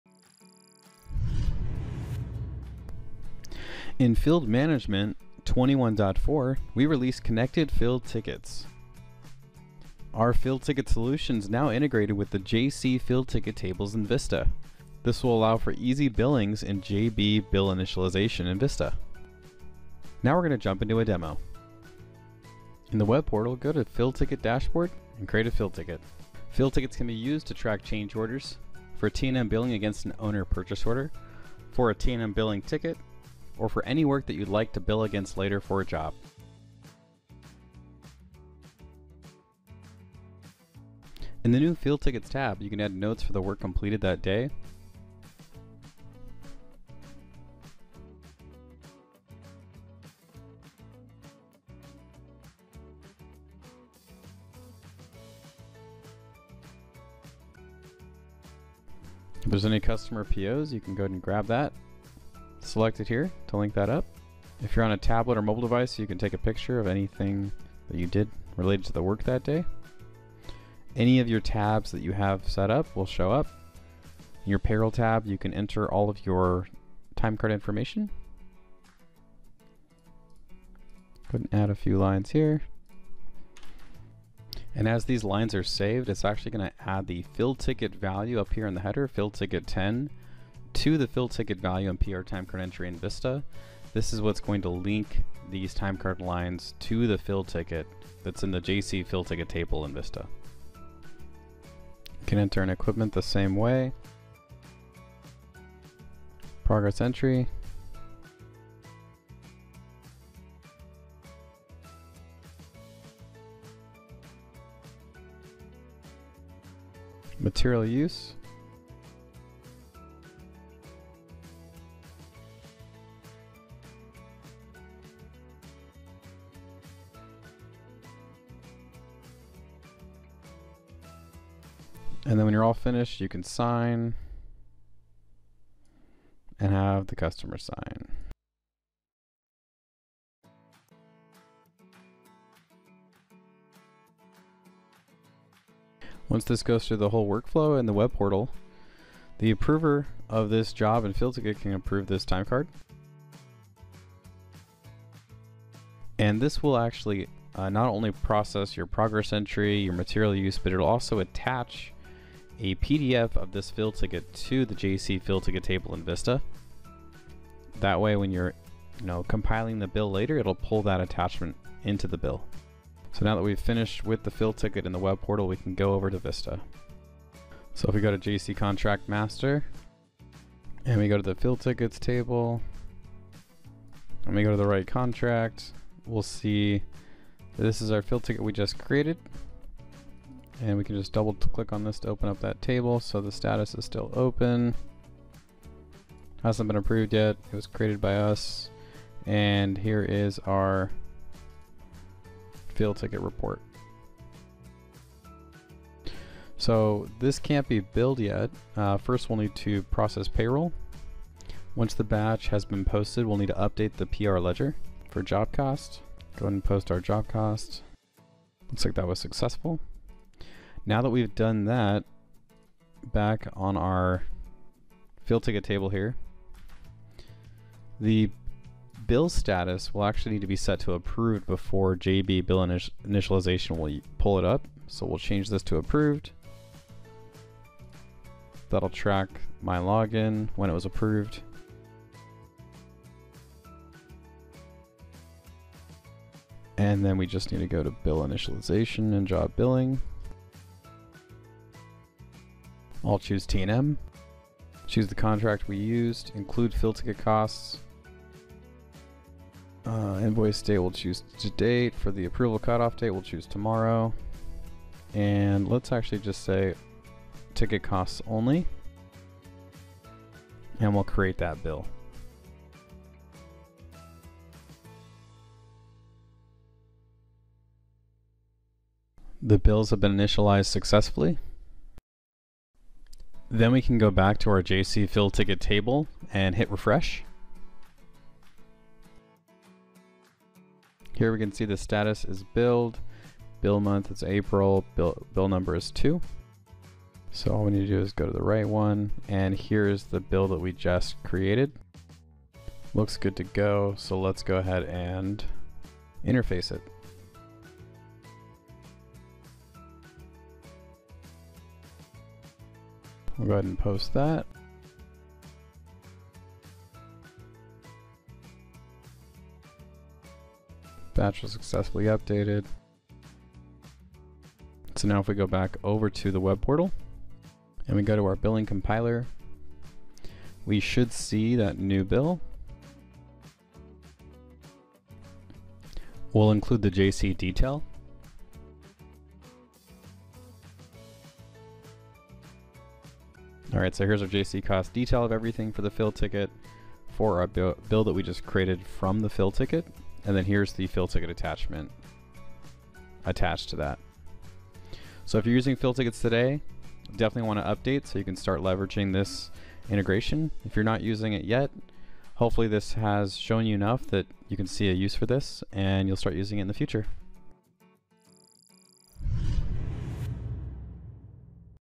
VIEWPOINT FIELD MANAGEMENT - FIELD TICKETS | Trimble Viewpoint | Labor, Equipment, Materials
Viewpoint Field Management now captures labor, equipment, and materials in the field. In the office, this construction solution allows the AP and Billing departments to create bills from approved job costs.